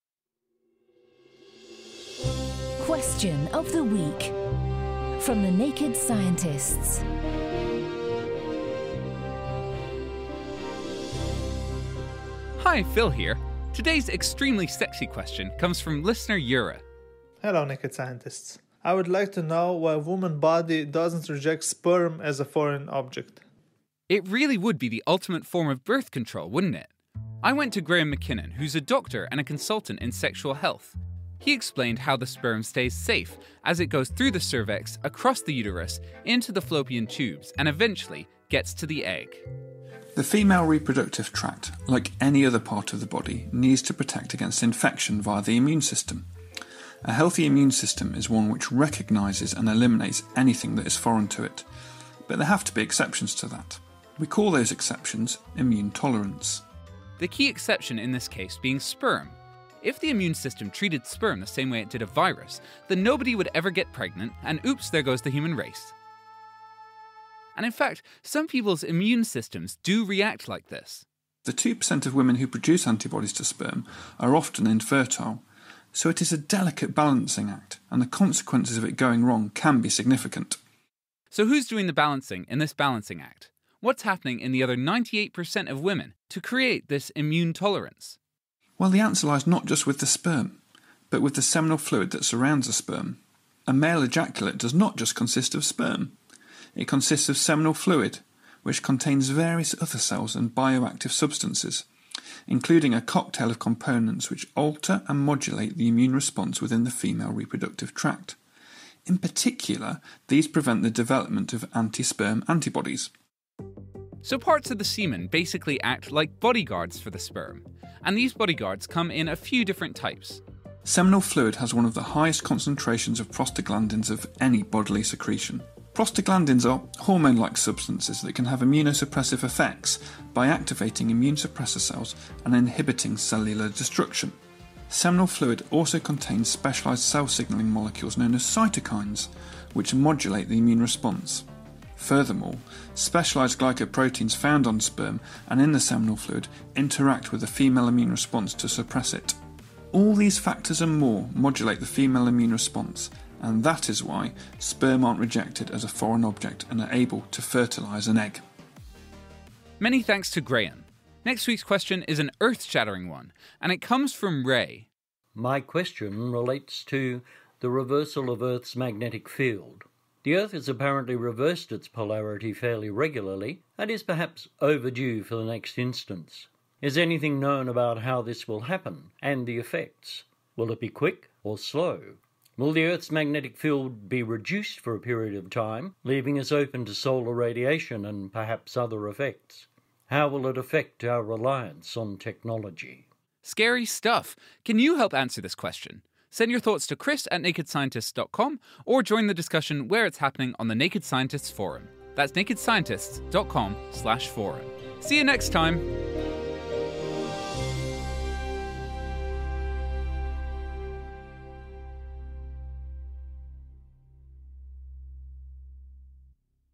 Why don't women's bodies reject sperm?
Jure asked us "Why doesn't a woman's body reject sperm as a foreign object?" Phil Sansom sought out the answer, by speaking to doctor and sexual health consultant Graham McKinnon... Like this podcast? Please help us by supporting the Naked Scientists